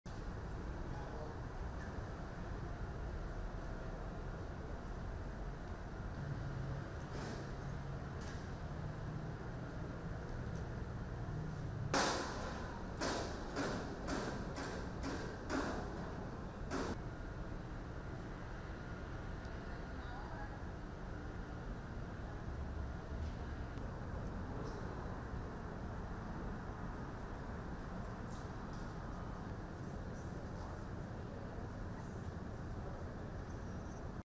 Miami: les électeurs se rendent aux urnes pour le vote anticipé
En Floride, les gens se rendent aux urnes pour le vote anticipé des élections de mi-mandat, autorisé par correspondance ou en personne dans la plupart des Etats américains. IMAGES